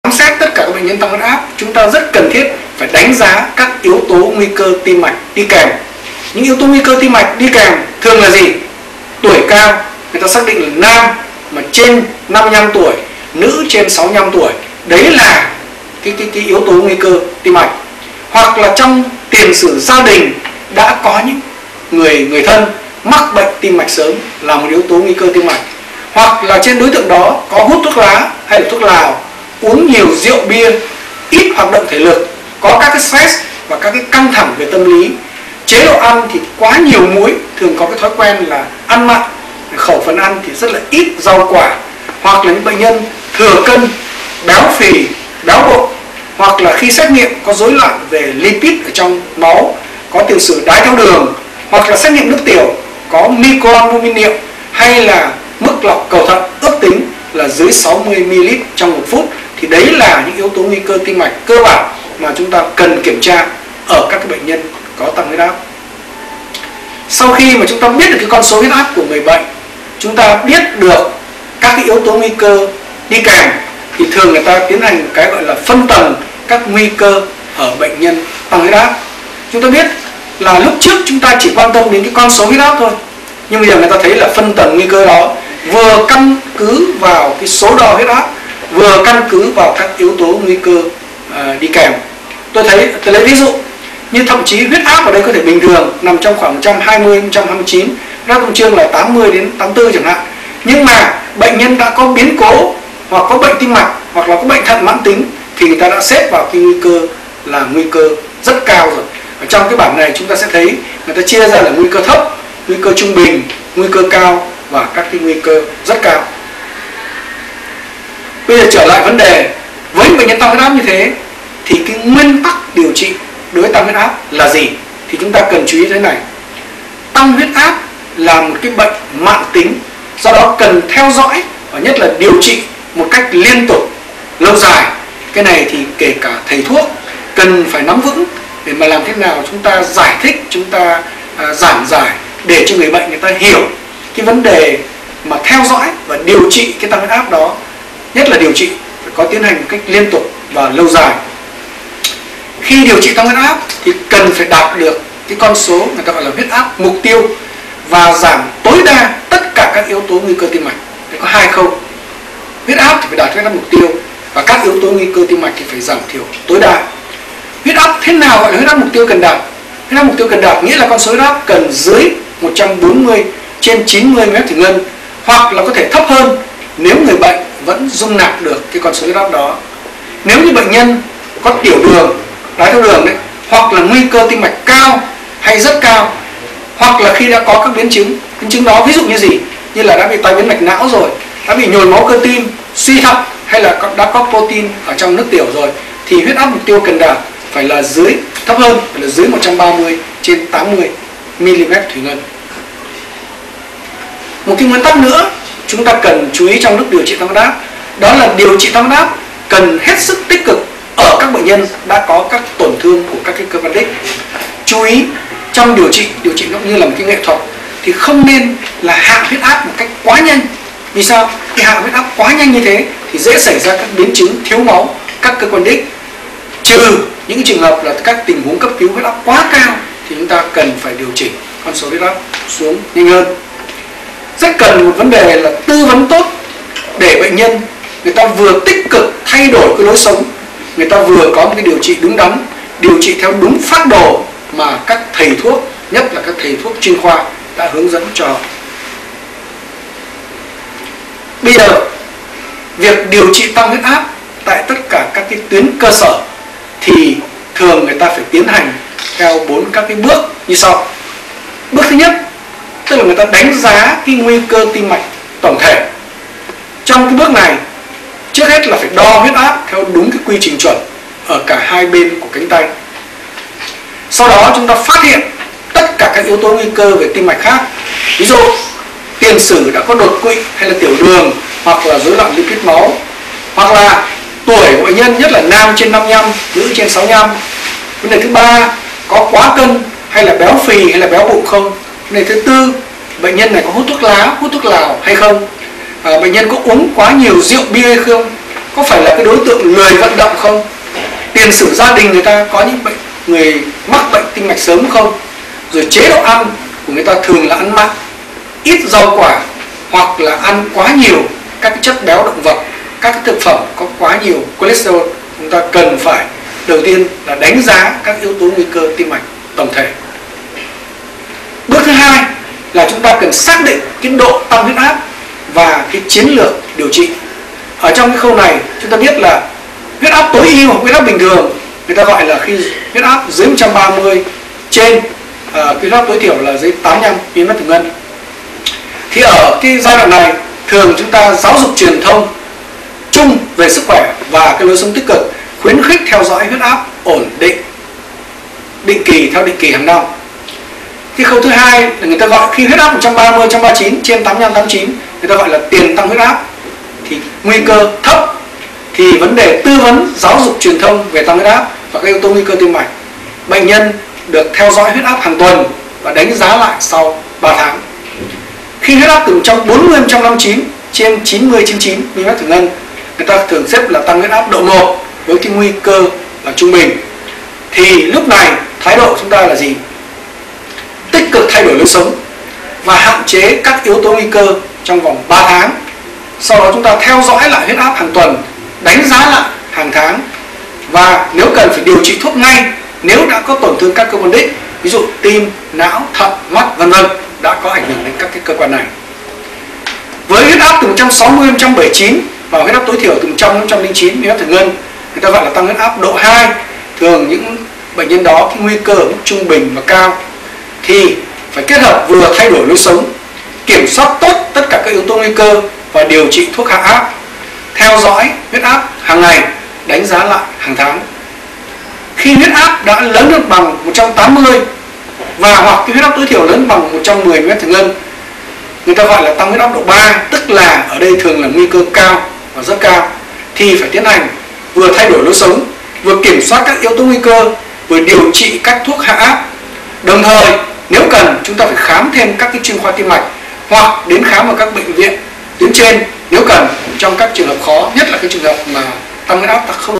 Hướng dẫn chẩn đoán và điều trị tăng huyết áp - P2
Bài giảng Hướng dẫn chẩn đoán và điều trị tăng huyết áp của Giáo sư Nguyễn Lân Việt - Dự án phòng, chống tăng huyết áp